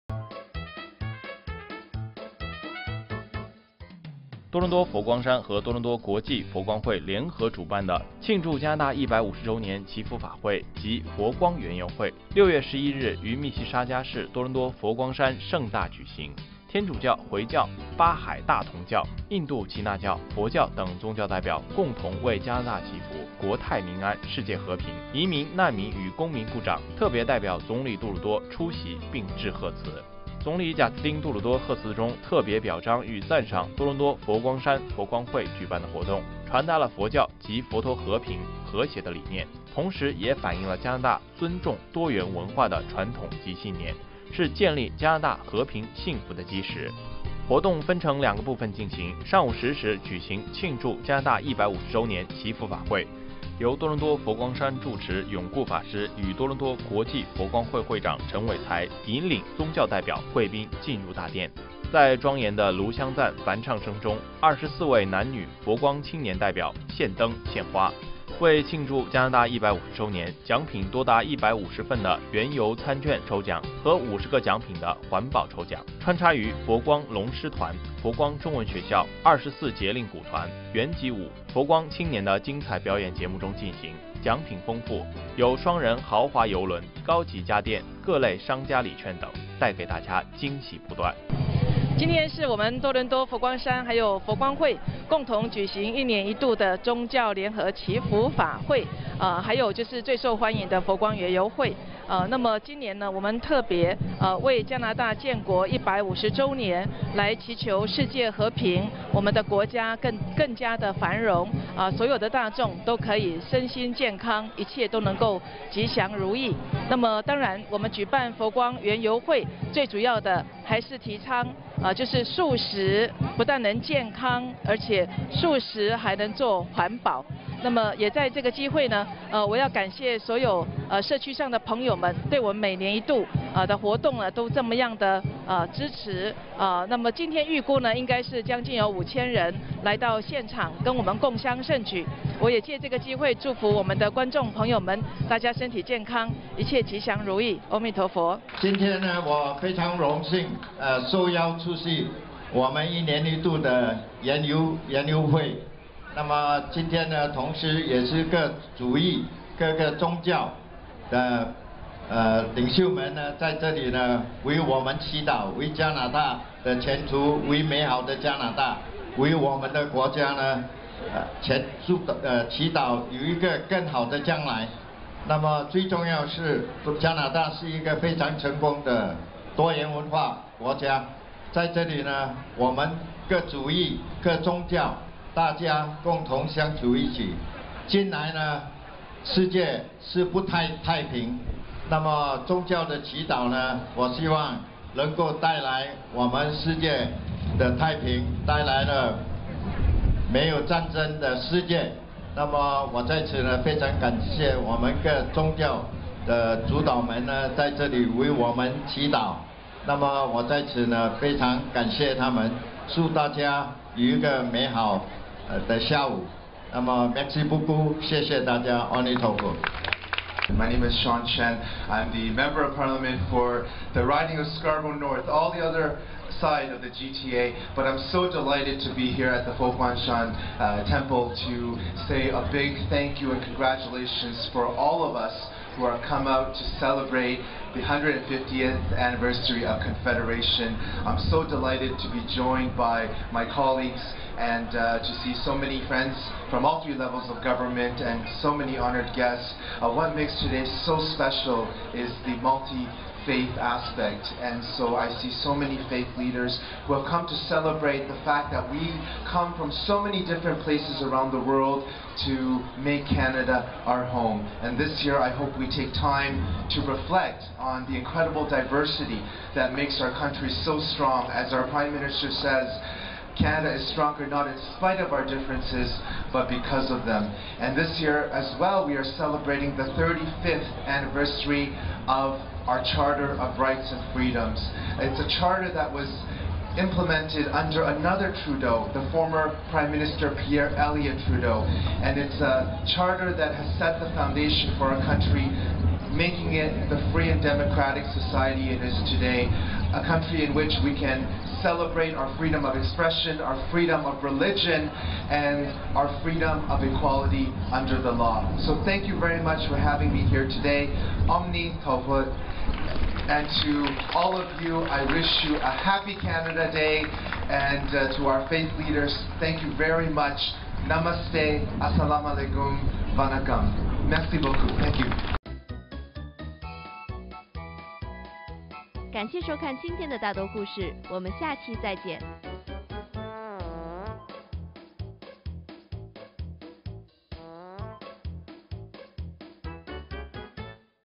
0616 多倫多佛光山慶祝加拿大150週年祈福法會暨佛光園遊會
0616 多倫多佛光山慶祝加拿大150週年祈福法會暨佛光園遊會
Toronto Weekly